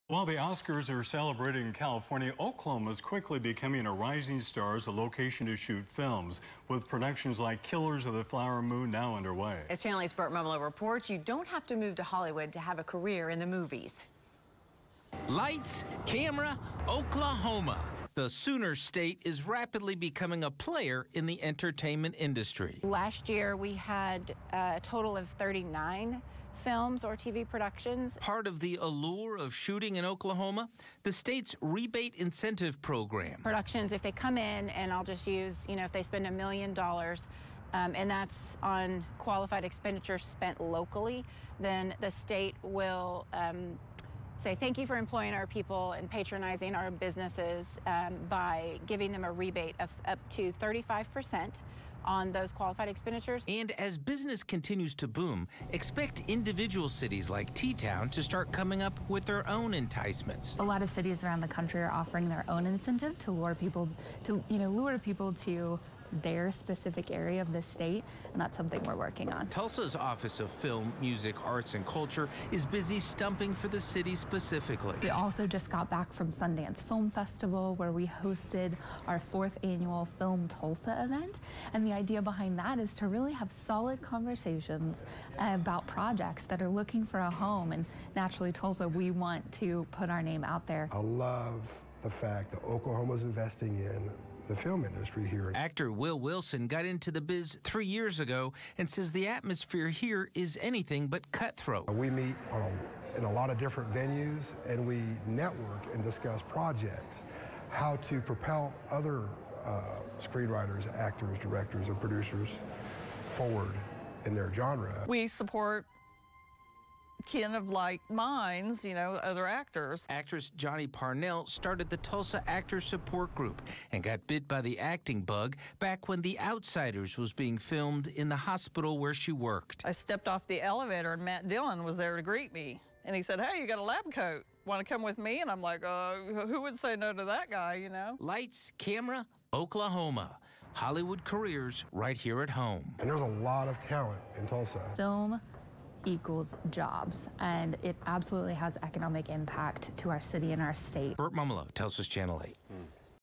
Oklahoma becoming rising star in movie industry
While the Oscars are celebrated in California, Oklahoma is quickly becoming a rising star as a location to shoot films, with productions like “Killers of the Flower Moon” currently underway.

Lights, camera, Oklahoma!

The Sooner state is rapidly becoming a player in the entertainment industry.

Channel 8's Burt Mummolo reports.